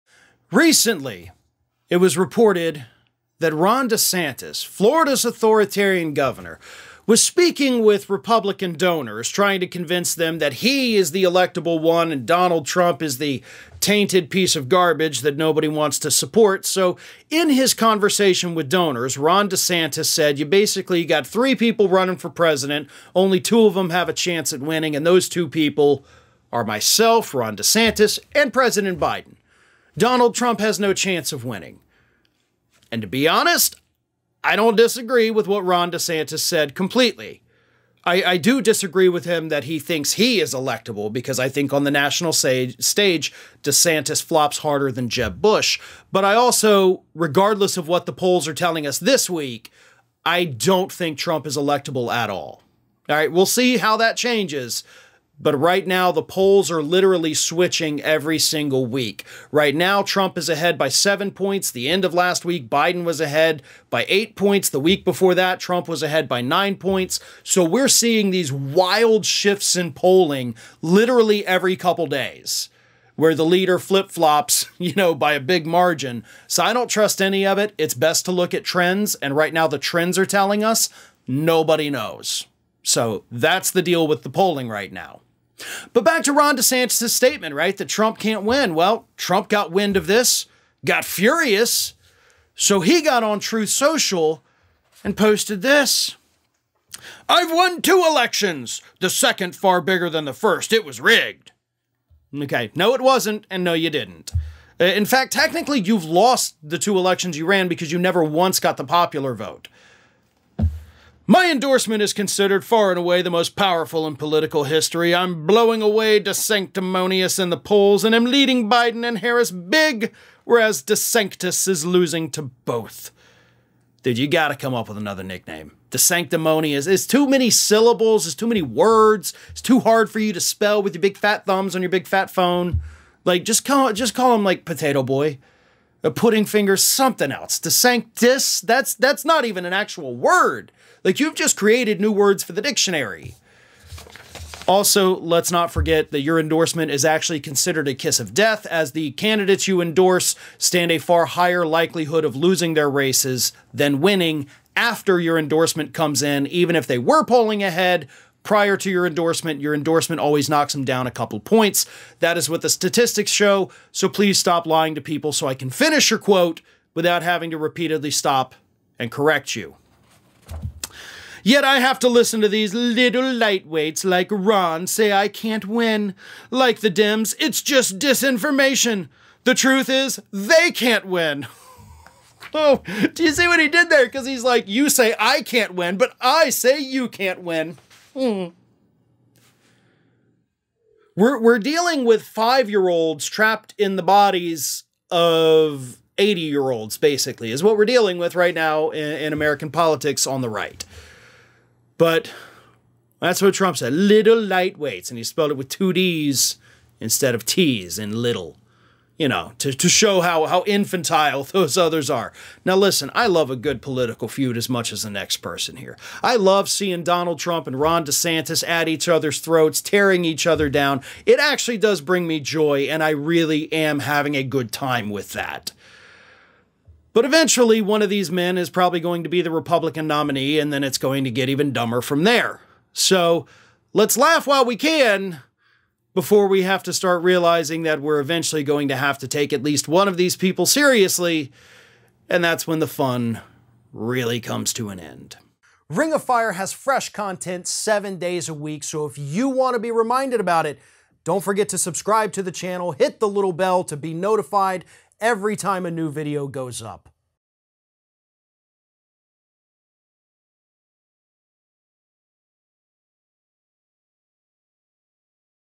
Trump Brutally Mocks DeSantis As 'Liddle Lightweight' As Feud Escalates
Donald Trump got on Truth Social on Monday to take a shot at Ron DeSantis after DeSantis recently told a group of donors that Trump doesn't stand a chance of beating President Biden. This infuriated Trump, and he used his social media platform to slam DeSantis as a "liddle lightweight," employing the spelling that a child would use to show how not afraid of DeSantis he is. But in every attack, Trump shows his fear, as Ring of Fire's Farron Cousins explains.

Recently it was reported that Ron DeSantis, Florida's authoritarian governor, was speaking with Republican donors, trying to convince them that he is the electable one, and Donald Trump is the tainted piece of garbage that nobody wants to support. So in his conversation with donors, Ron DeSantis said, you basically got three people running for president. Only two of them have a chance at winning. And those two people are myself, Ron DeSantis and President Biden. Donald Trump has no chance of winning. And to be honest, I don't disagree with what Ron DeSantis said completely. I I do disagree with him that he thinks he is electable because I think on the National S stage, DeSantis flops harder than Jeb Bush. But I also, regardless of what the polls are telling us this week, I don't think Trump is electable at all. All right. We'll see how that changes. But right now, the polls are literally switching every single week.

Right now, Trump is ahead by seven points. The end of last week, Biden was ahead by eight points. The week before that, Trump was ahead by nine points. So we're seeing these wild shifts in polling literally every couple days where the leader flip flops, you know, by a big margin. So I don't trust any of it. It's best to look at trends. And right now the trends are telling us nobody knows. So that's the deal with the polling right now. But back to Ron DeSantis statement, right, that Trump can't win. Well, Trump got wind of this, got furious. So he got on truth social and posted this. I've won two elections the second far bigger than the first it was rigged. Okay? No it wasn't. And no you didn't. Uh, in fact, technically you've lost the two elections you ran because you never once got the popular vote.

My endorsement is considered far and away the most powerful in political history. I'm blowing away des sanctimonious in the polls, and I'm leading Biden and Harris big. Whereas DeSantis is losing to both. Dude, you gotta come up with another nickname. Des Sanctimonious is too many syllables. There's too many words. It's too hard for you to spell with your big fat thumbs on your big fat phone. Like, just call, just call 'em like potato boy, a pudding finger, something else to sank this. That's, that's not even an actual word. Like you've just created new words for the dictionary. Also, let's not forget that your endorsement is actually considered a kiss of death as the candidates you endorse stand a far higher likelihood of losing their races than winning after your endorsement comes in, even if they were polling ahead prior to your endorsement, your endorsement always knocks 'em down a couple points. That is what the statistics show. So please stop lying to people so I can finish your quote without having to repeatedly stop and correct you. Yet. I have to listen to these little lightweights, like Ron say, I can't win like the dims. It's just disinformation.

The truth is they can't win. Oh, do you see what he did there? Because he is like, you say, I can't win, but I say you can't win. Hmm. We're, we're dealing with five year olds trapped in the bodies of 80 year olds, basically is what we're dealing with right now in, in American politics on the right. But that's what Trump said, little lightweights, and he spelled it with two Ds instead of Ts and little, you know, to, to show how, how infantile those others are.